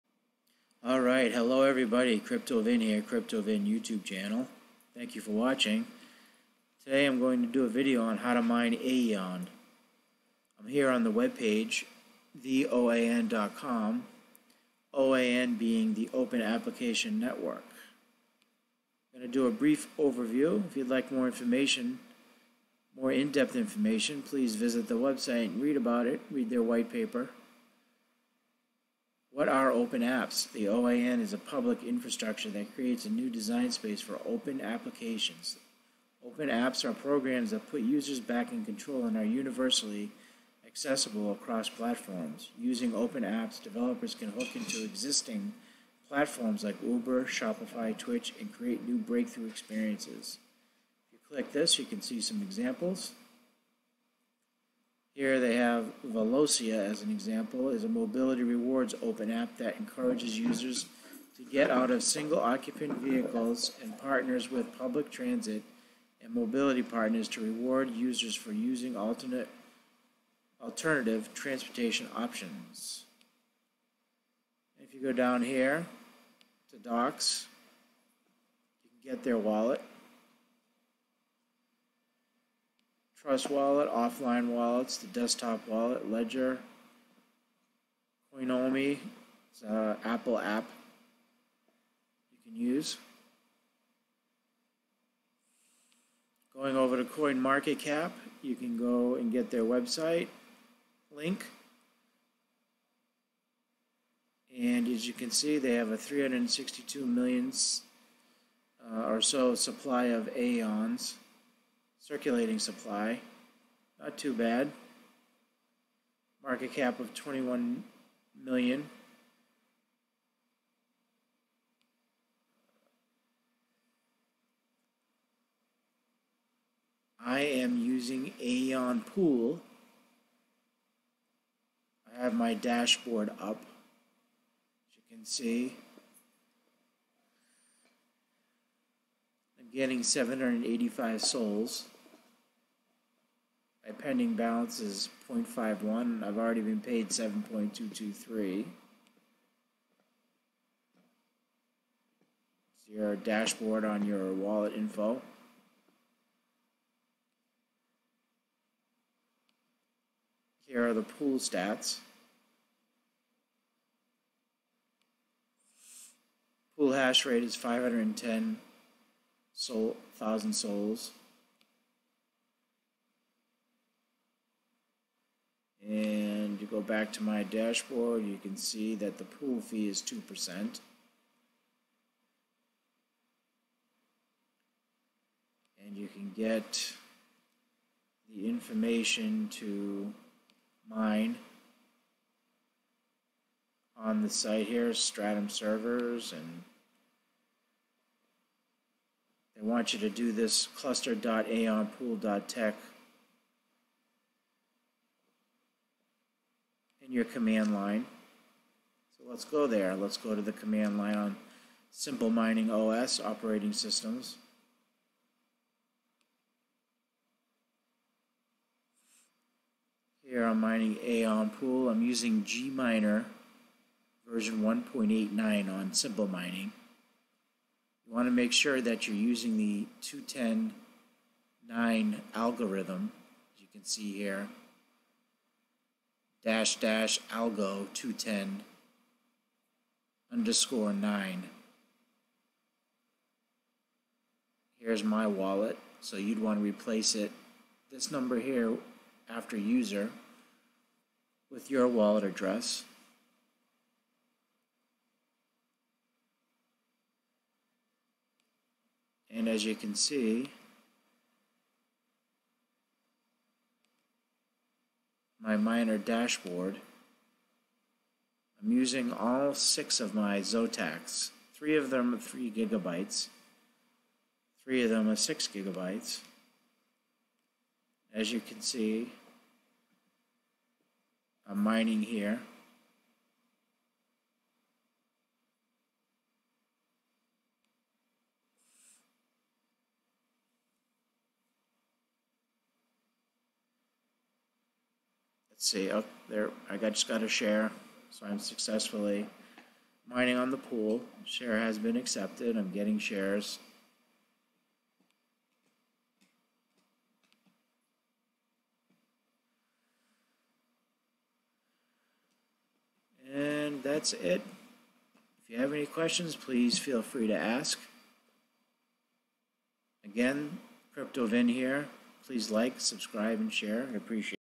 How to GPU Mine Aion (AION) the Open Application Network and Review
This is a brief overview of AION and a tutorial on how to mine it using Simplemining OS. I hope this helps save you some time and you can quickly get your rig (s) up and running. See below for the command line, replace my wallet public key number with your wallet public key number after --user

--algo 210_9 --pers AION0PoW --server cluster.aionpool.tech --port 2222 --user 0xa0acc0422a00d1667b911c1615e4a781d397252a10839aee9408adb85d81add6 --pass x --api 3333

How about a beer?
BTC bc1qjpxr4axhfz8avpj8t82sdyurunxnkg5gvkfrs0
ETH 0x829BB200f8C9d95AdA2511F89Ee010C14257C545
LTC LU6vAQ226mHeY2VQaiPTUVJeKqzCuQzoY8